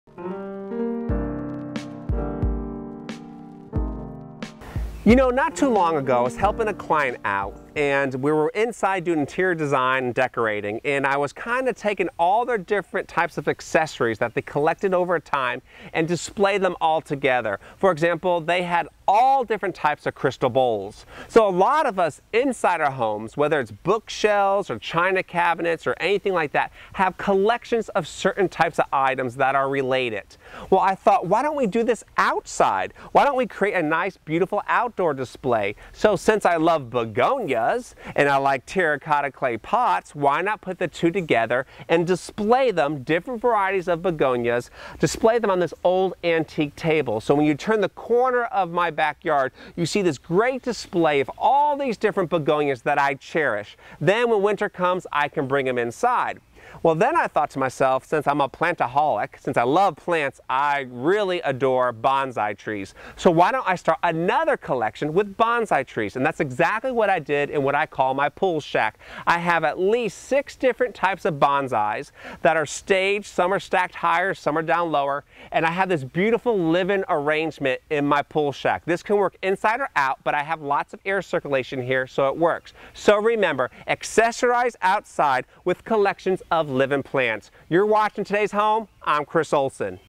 Today's Home with Chris H. Olsen - Collect Living Plants
You know not too long ago I was helping a client out and we were inside doing interior design decorating and I was kinda taking all their different types of accessories that they collected over time and display them all together.  For example they had all different types of Crystal Bowls.  So a lot of us inside our homes, whether it’s bookshelves or China Cabinets or anything like that have collections of certain types of items that are related.  Well I thought - why don’t we do this outside?  Why don’t we create a nice beautiful outdoor display?  So since I love Begonias - and I like Terra Cotta clay pots - why not put the two together and display them - different varieties of Begonias - display them on this old antique table.  So when you turn the corner of my backyard you see this great display of all these different Begonias that I cherish.  Then when winter comes I can bring them inside.  Well then I thought to myself - since I am a plantoholic - since I love plants - I really adore Bonsai trees.  So why don’t I start another collection with Bonsai Trees - and that is exactly what I did in what I call my pool shack.  I have at least six different types of Bonsai’s that are staged.  Some are stacked higher some are down lower and I have this beautiful living arrangement in my pool shack.  This can work inside or out but I have lots of air circulation here so it works.  So remember - accessorize outside with collections of living plants.  You’re watching Today’s Home - I’m Chris Olsen.